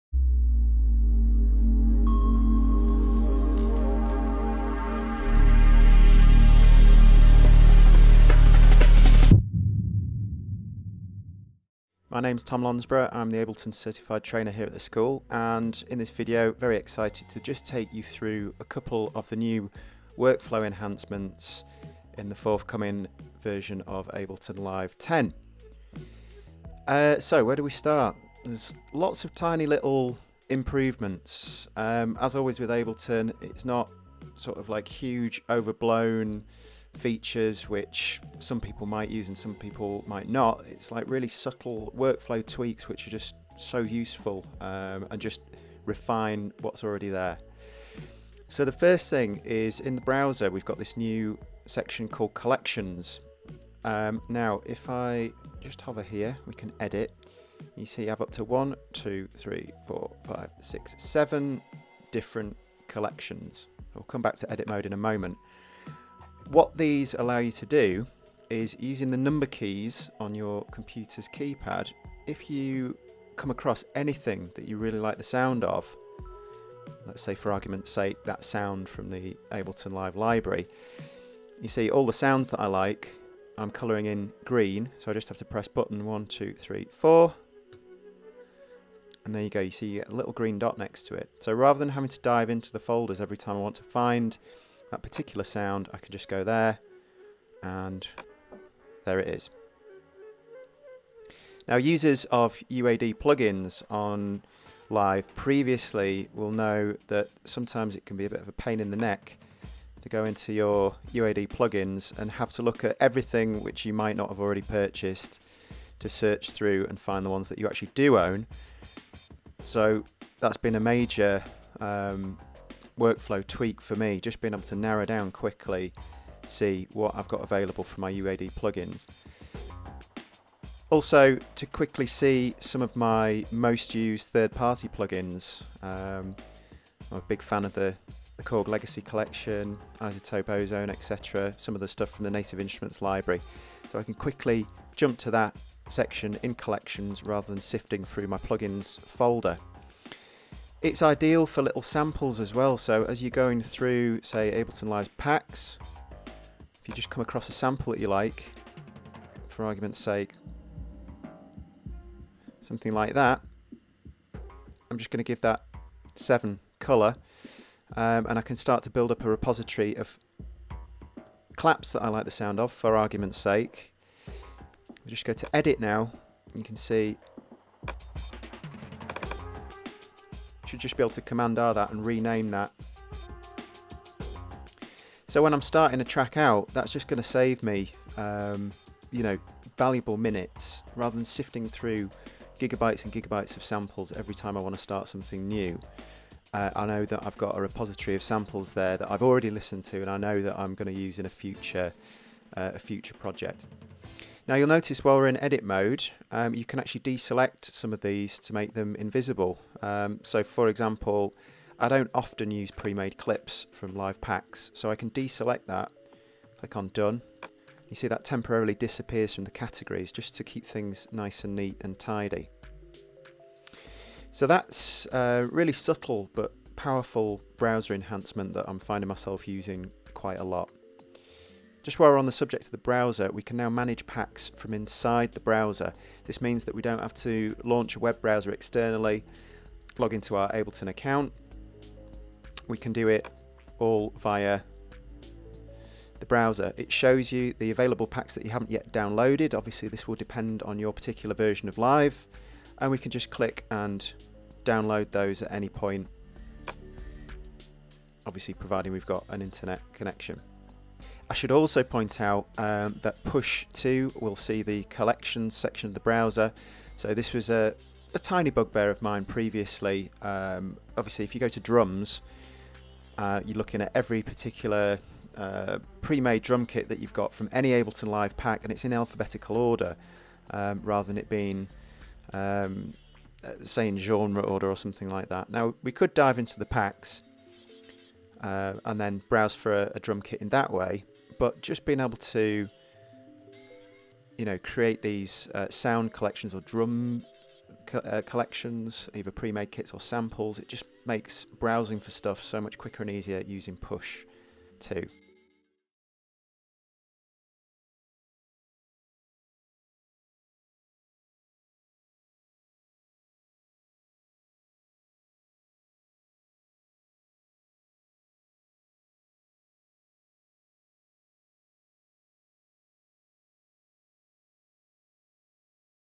Ableton Live 10 First Look: Browser Enhancements
Ableton Certified Head Lecturer Tom Lonsborough takes a first look Live 10.

In this video we cover the Browser Enhancements.